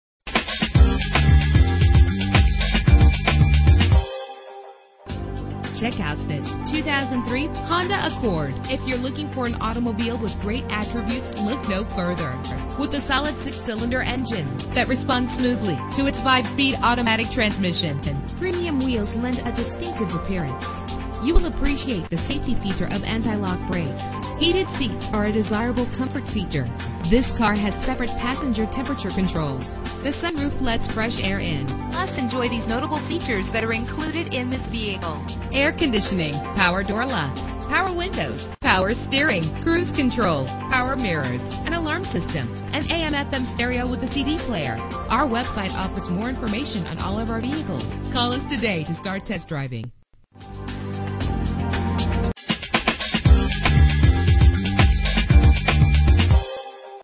2003 Honda Accord - Bellevue WA
Year: 2003
Make: Honda
Model: Accord
Trim: 3.0 EX
Engine: 3.0 liter 6 cylinder 24 valve
Transmission: 5-Speed Automatic
Color: Gray
Mileage: 70984
Address:
15000 SE Eastgate Way